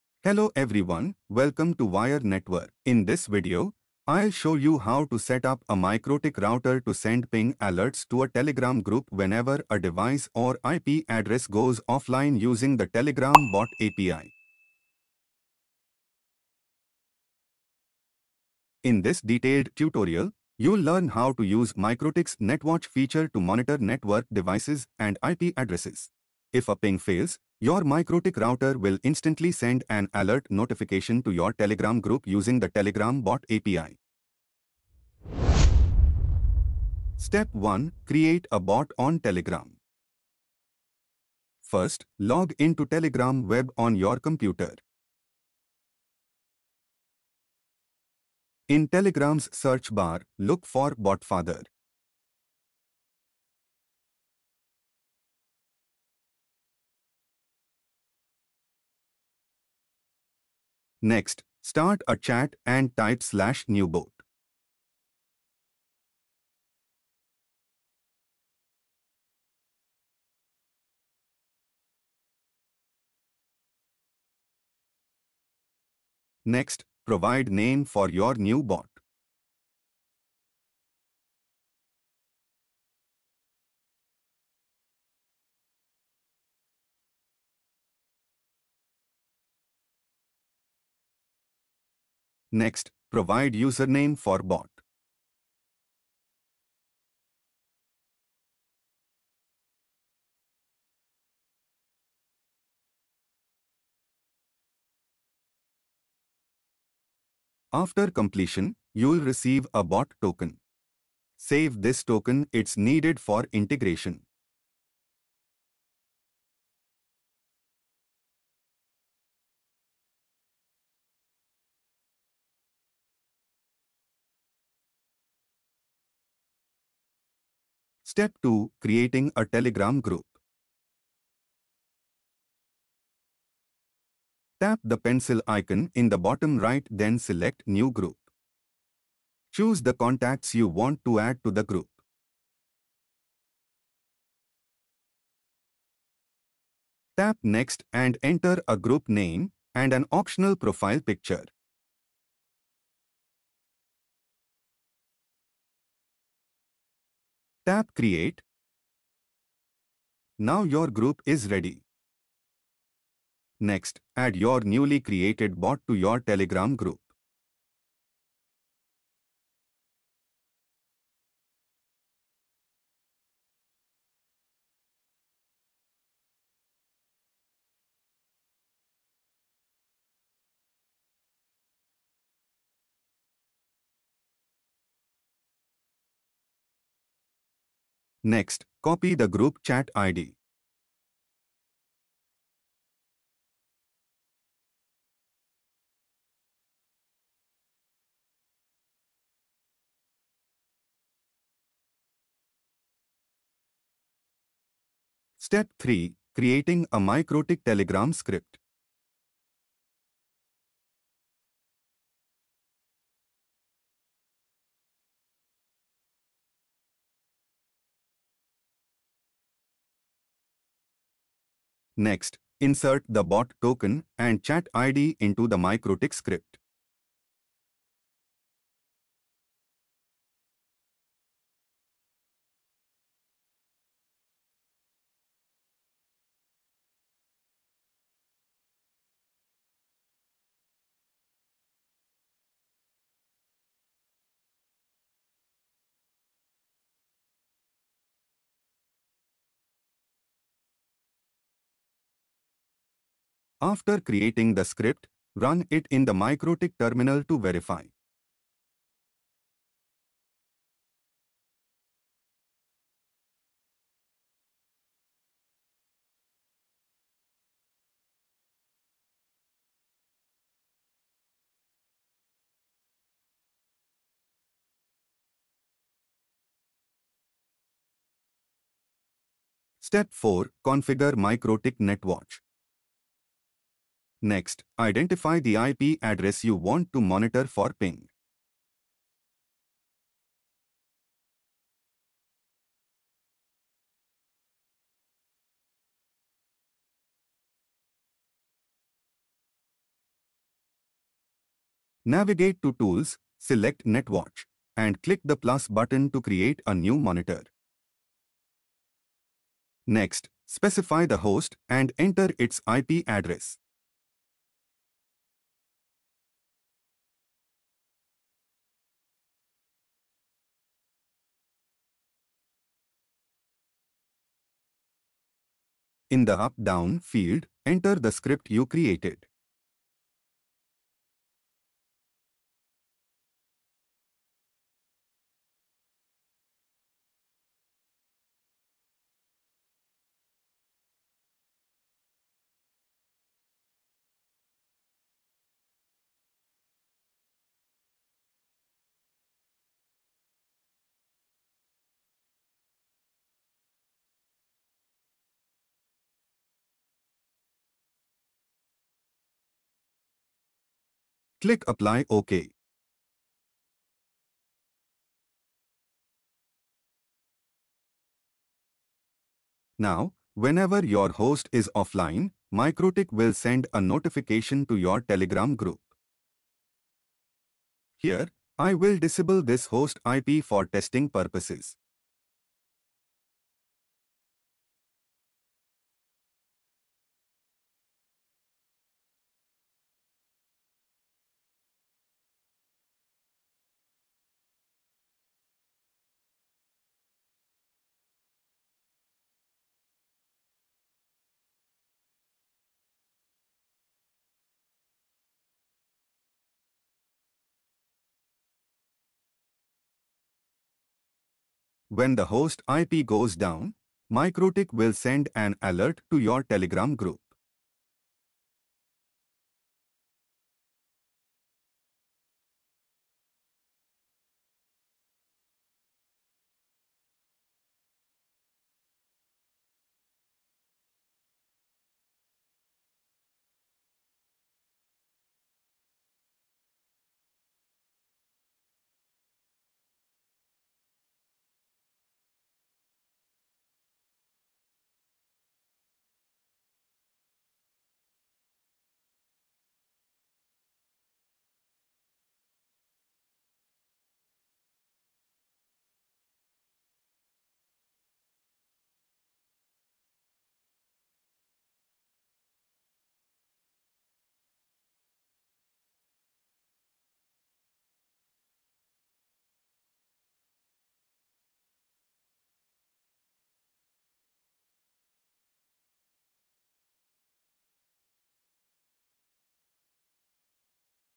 MikroTik Netwatch : Instant Telegram Alerts for IP Down | RouterOS v7
Want to receive instant Telegram notifications when your IP goes down? In this tutorial, we’ll show you how to set up MikroTik Netwatch to send real-time Telegram alerts whenever an IP becomes unreachable.

🔹 What You'll Learn:
✅ Setting up Telegram bot for automated alerts
✅ Setting up MikroTik Netwatch
✅ Ensuring 24/7 network uptime monitoring

💡 Whether you're an IT admin, network engineer, or MikroTik enthusiast, this guide will help you automate downtime alerts and stay ahead of network issues!